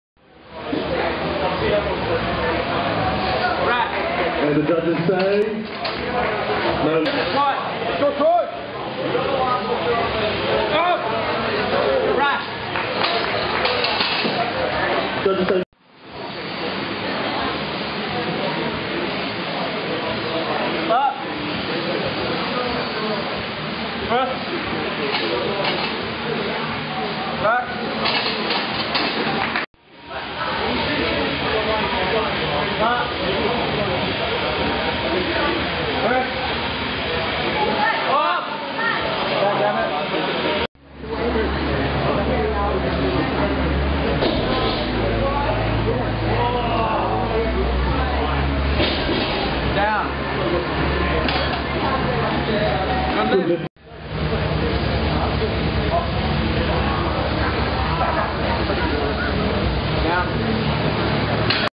Tenacity #2 Jan 19th Sunday 2014 Troy Conley Total 340kg @ 97kg
Squat = 60kg / 90kg / 90kg
Bench = 60kg / 80kg / 90kg
Deadlift =  140kg / 170kg / 180kg